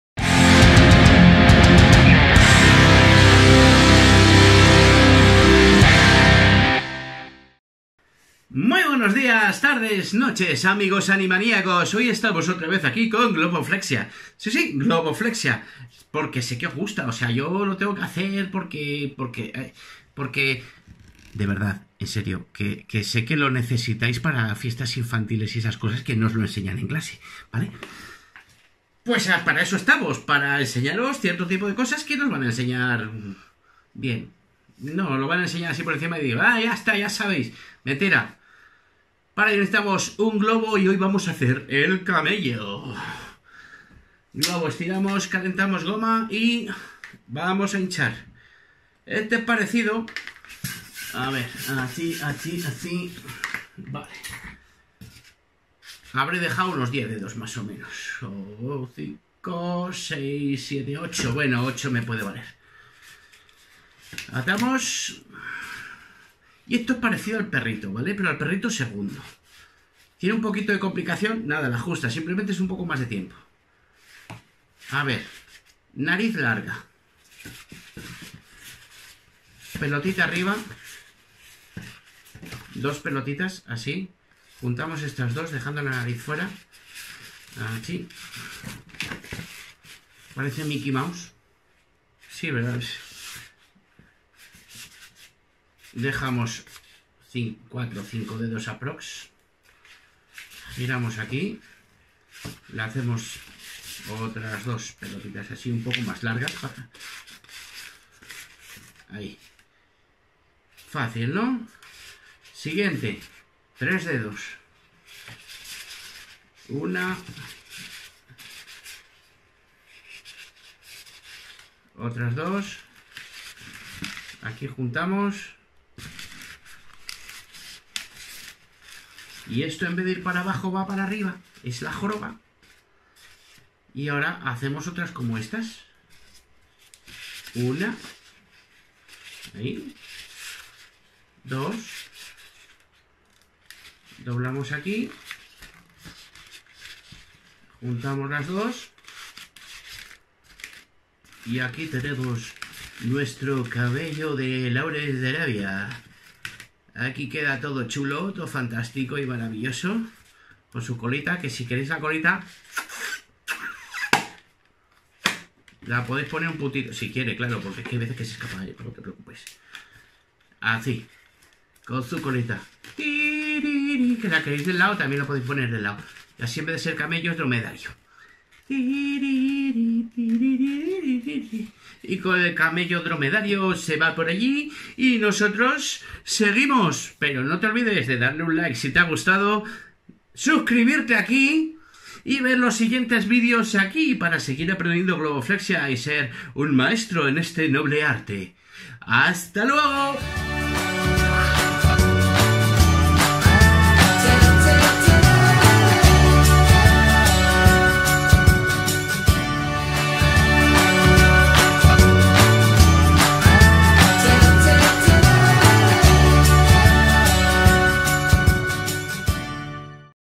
✔ Curso de Globoflexia - Camello 01 [Animación Profesional] ⭐⭐⭐⭐⭐ En Español 🇪🇸 #7
Curso online de Globoflexia para animadores, profesores o personas que quieran aprender.
Impartido por José Luis Soladana Mago y animador profesional con más de 10 años de experiencia en animación.